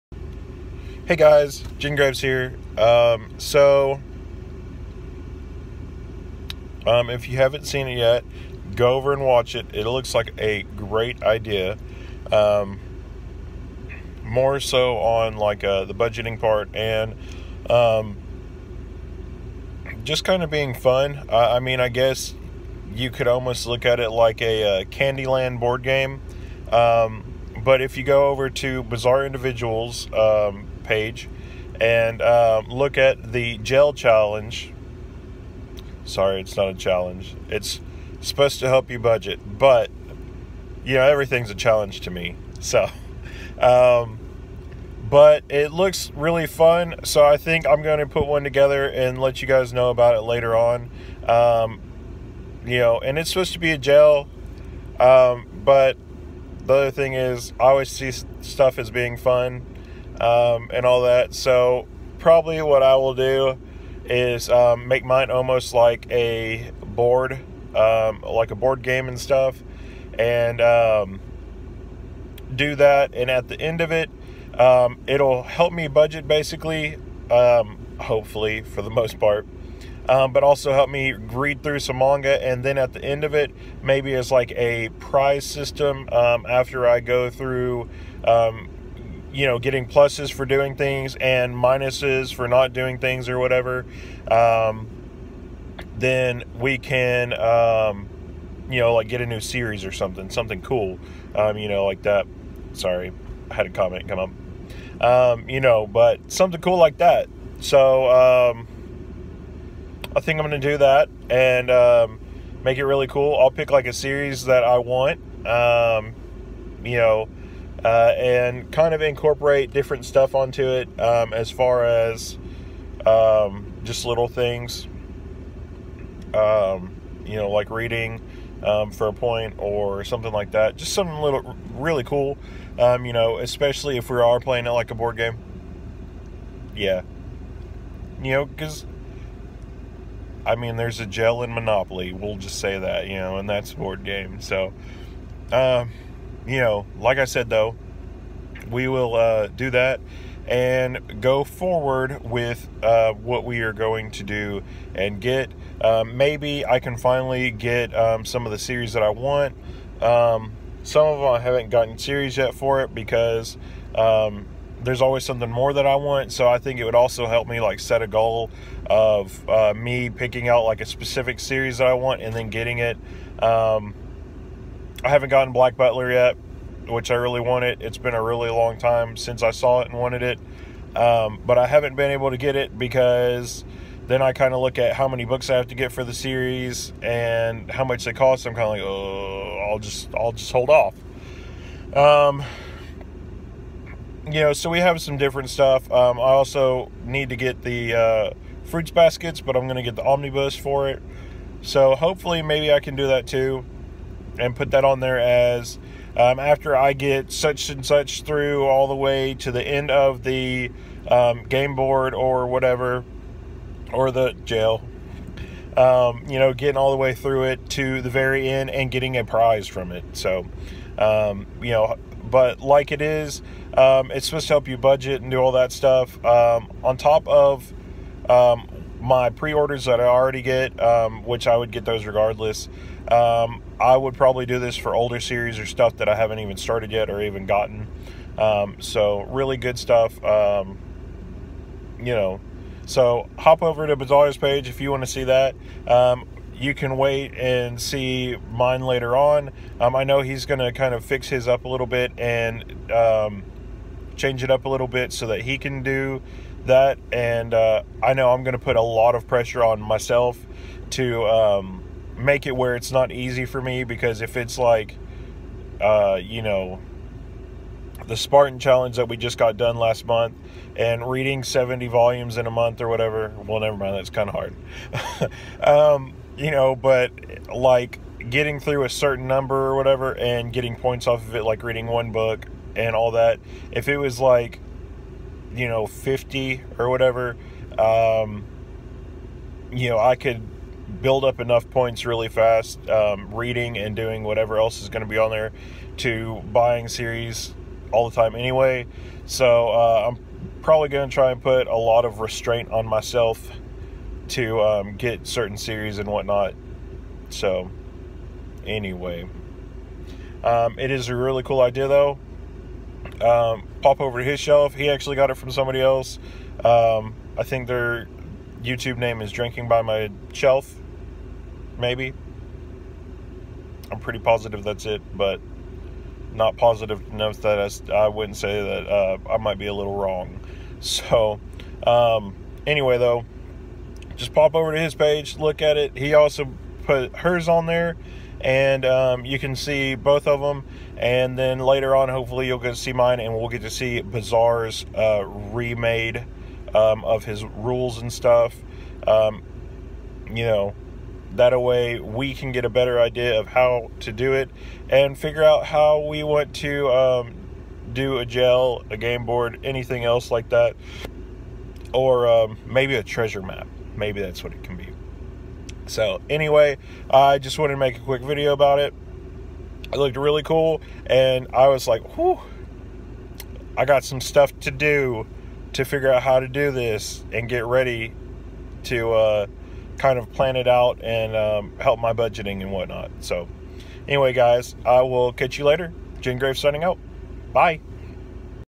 Manga Talk: Budget Jail or Budget Game Board
This is just something Bizarre Individual put up I though was cool and may try and do this.